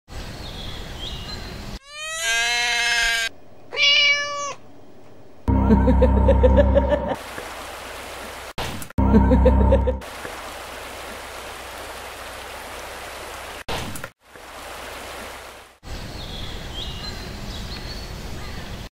Amy - Bad Cat
Introduction to Animation Class at Bonsor Community Centre October 2015.